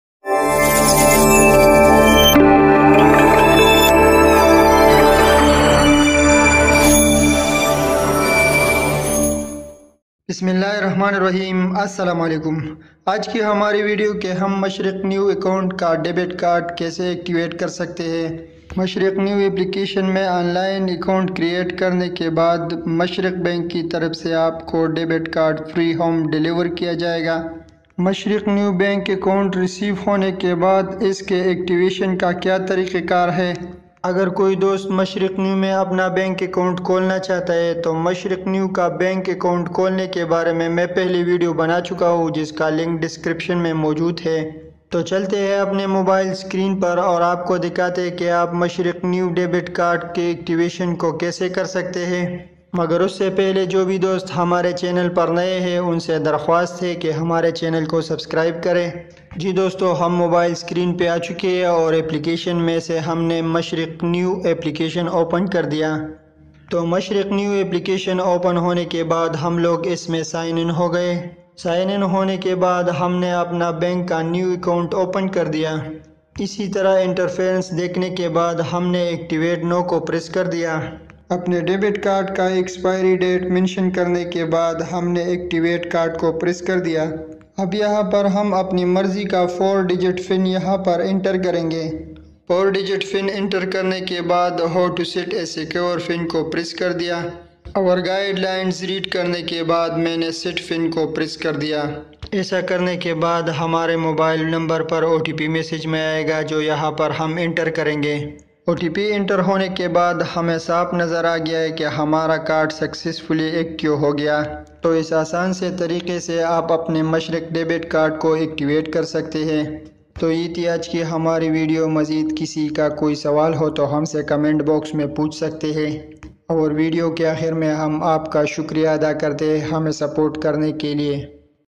How To Activate Mashreq Neo Debit Card Online With Mobile in UAE
This video is all about to Activate Mashreq Neo Debit Card Online With Mobile in UAE. below steps will help you to activate mashreq neo debit card for more information watch the full video and not skip.

Card Activation/Pin Generation
You can activate your Mashreq Al Islami Debit Card in any of the following ways: SMS 'DCA' [space]Last four digits of your Card to 4250 through the registered mobile number e.g., DCA 3457. Log on to Mashreq Online.

Think for watching and supporting.